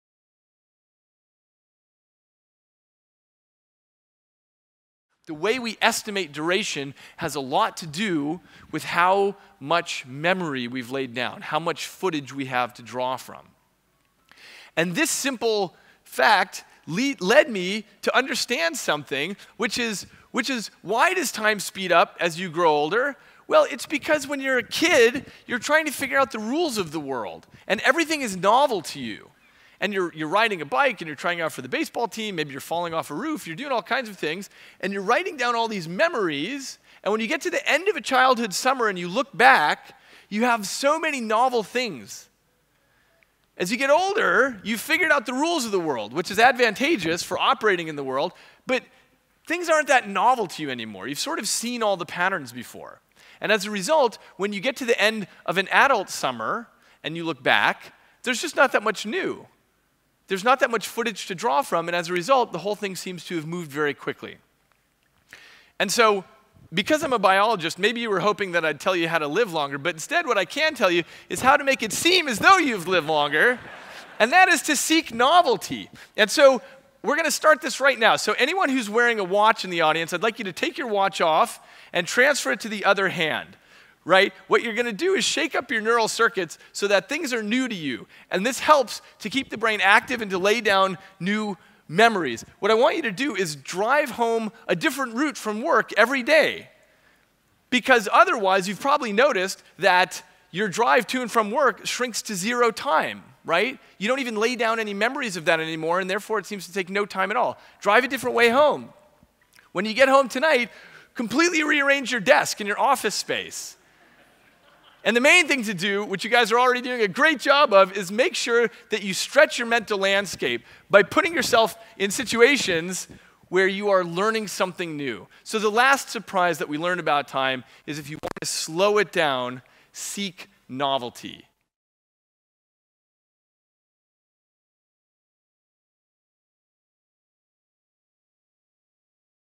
Novel Memories Slow Our Perception of Time
Best of FORA.tv 2012 Science -- Neuroscientist and bestselling author David Eagleman explains why time seems to go faster as we age, saying, "The way we estimate duration has a lot to do with how much memory we've laid down."